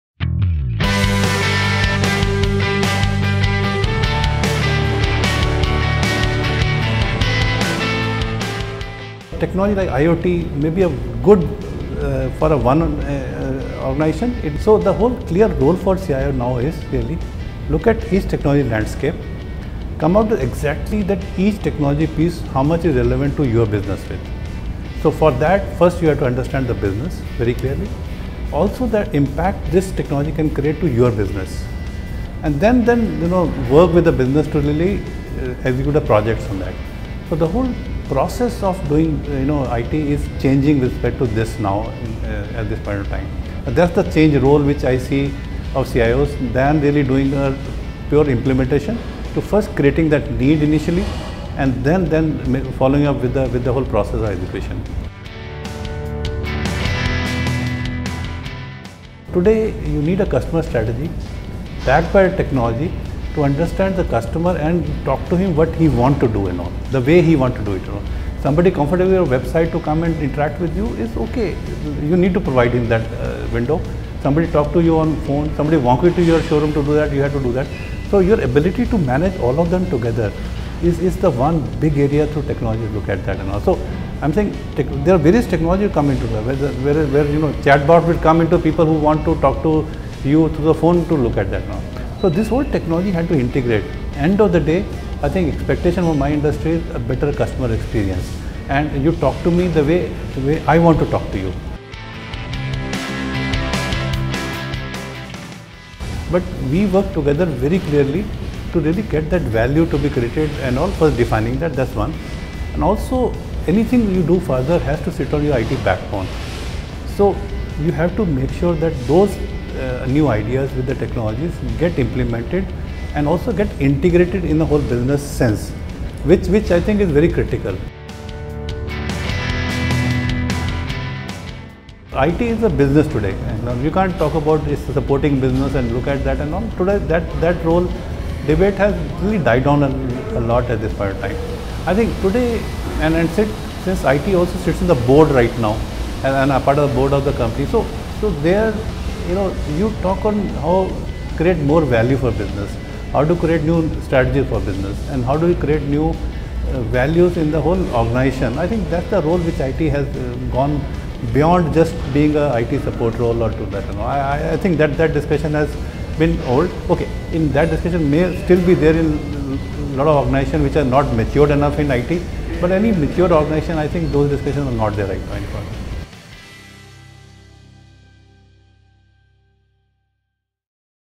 How the CIO of Maruti Suzuki India is Leading the Company into the Future
Rajesh Uppal, CIO & Executive officer(IT) , Maruti Suzuki India discusses how CIOs need to lead the technology and customer experience strategy of their enterprise to tackle the ever changing business environment.

To stay up to date on the enterprise tech world and know about our upcoming events go to our main